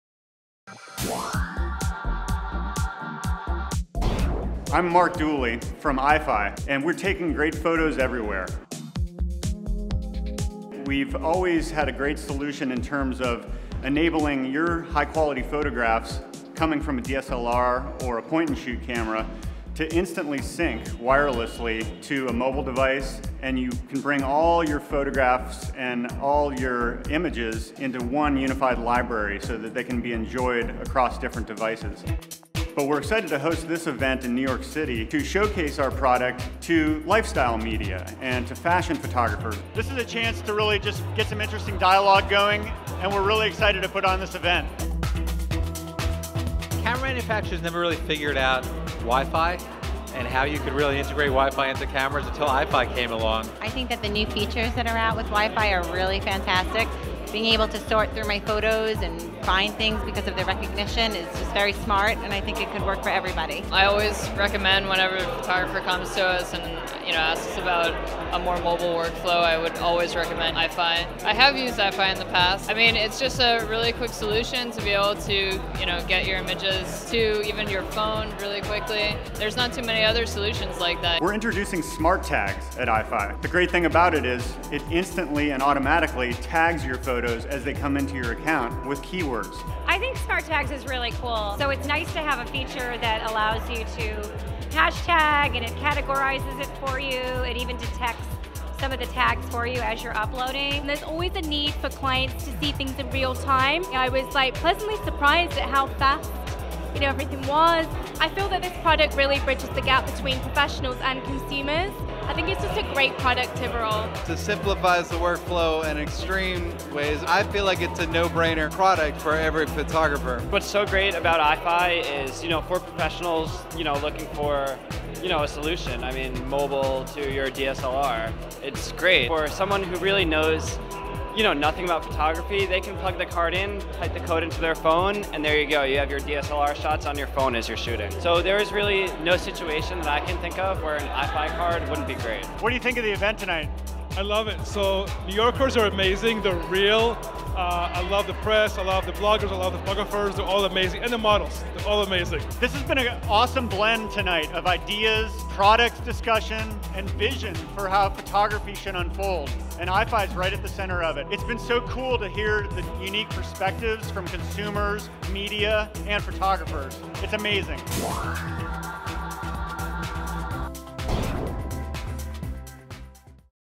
Eyefi Sizzle Video - NYC May 2015
Eyefi hosted a media event in New York's Chelsea District at Drift Studio, with Lara Jade, Jeremy Cowart and some of the most influential lifestyle photography bloggers and media.  Showcasing newest feature Smart Tags, attendees were treated to a lively evening of demos, photo stations and samples.